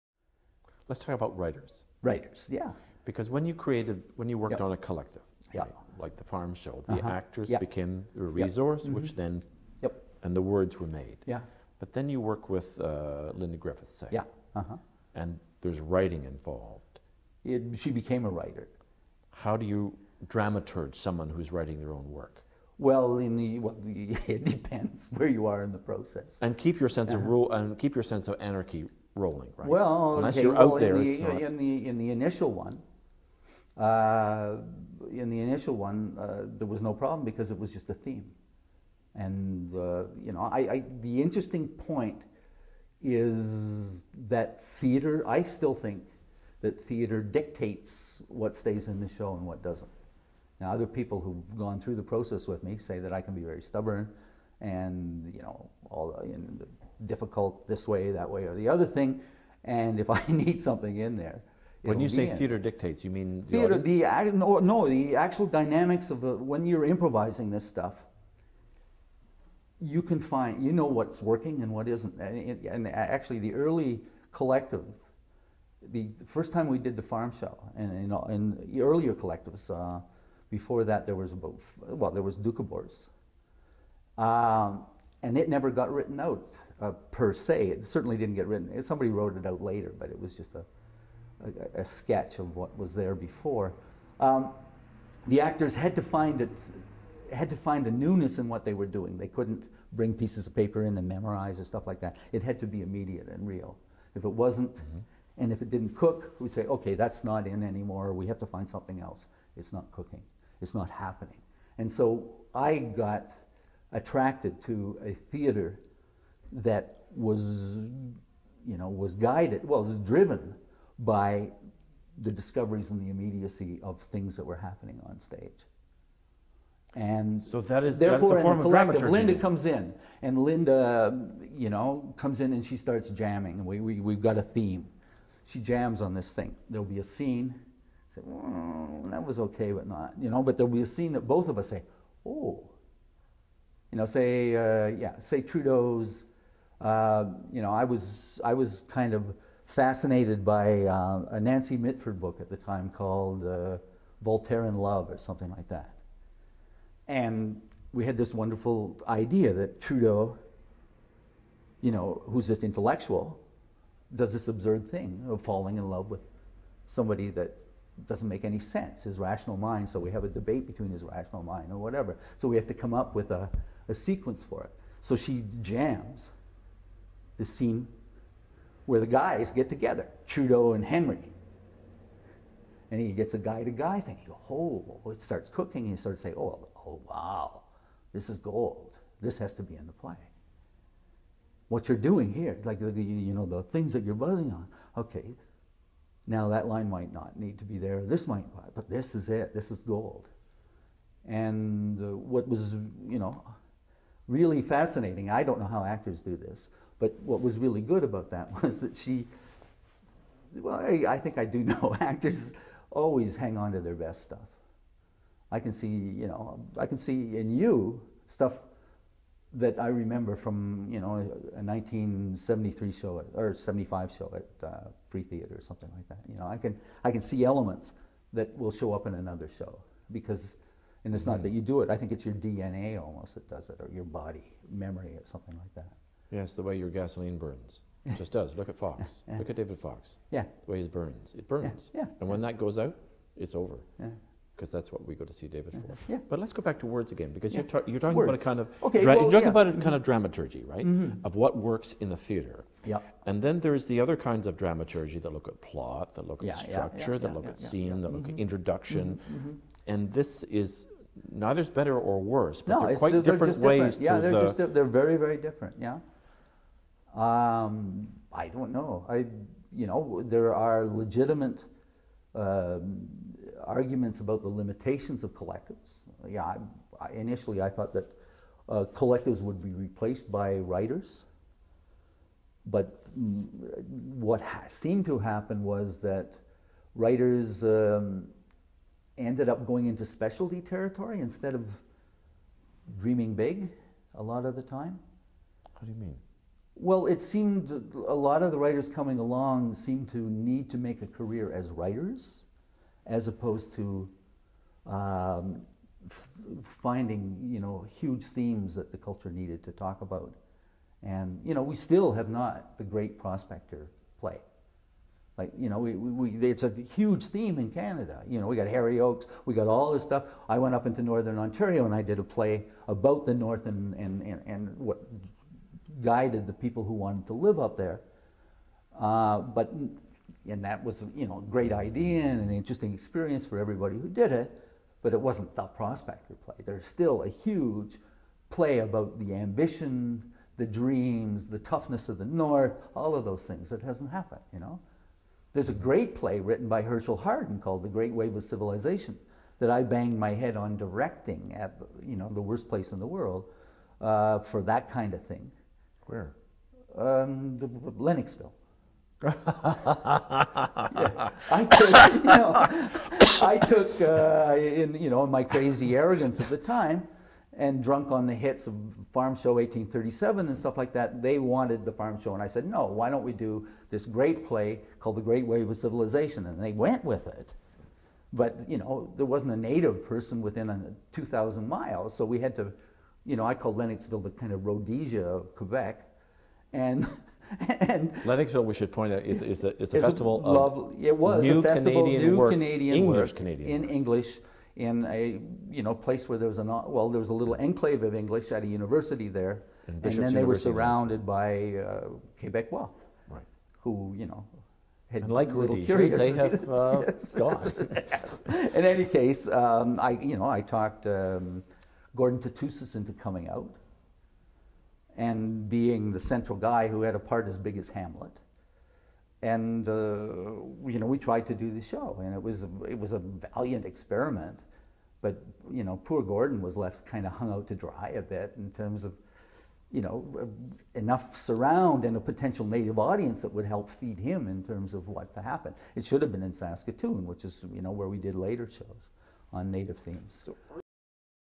Paul Thompson on writing for collective theatre (Part 17 of 21)
Paul Thompson on working through process, improvisation, and unscripted experimentation.

This interview was conducted by R.H. Thomson on January 25, 2007. Filming location courtesy of Theatre Pass Muraille.

This installment is made possible by a gift from John McKellar.